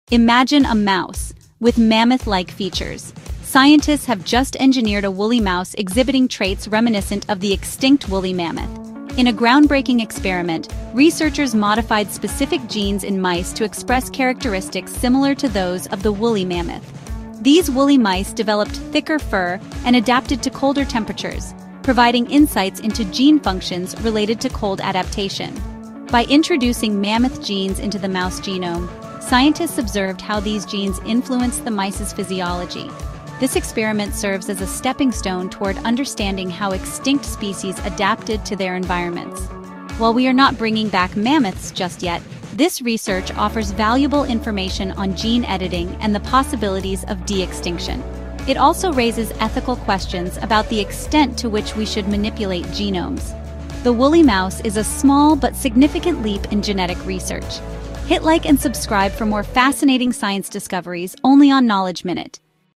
Scientists Create 'Woolly Mouse' with Mammoth-Like Traits
Scientists have engineered mice to express genes from the extinct woolly mammoth, resulting in 'woolly mice' with thicker fur and cold adaptations. This experiment sheds light on gene functions and the possibilities of de-extinction. Discover how this tiny creature could pave the way for future genetic breakthroughs.